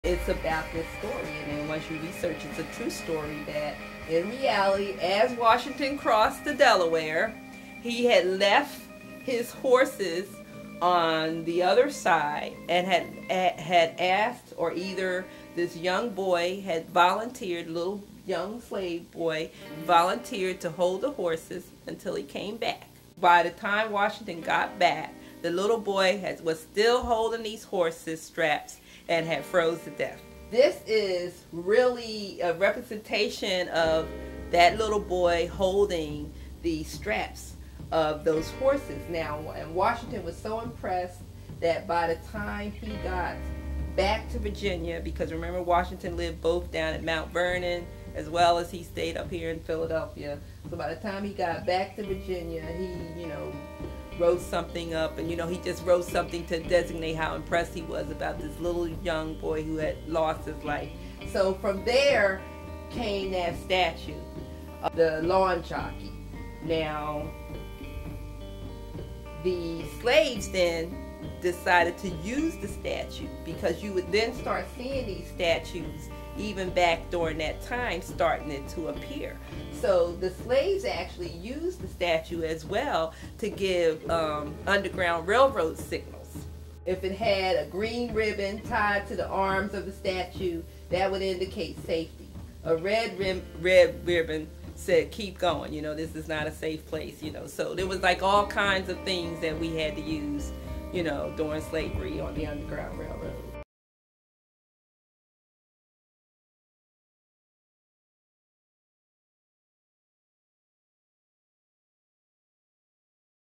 The True Story of Jocko - the Lawn Jockey
It's time you learned the true story.  This story is included in our larger traveling exhibits: African Americans during the Revolutionary War, the Little Legends, and Addy's War - Life of Children During and after Slavery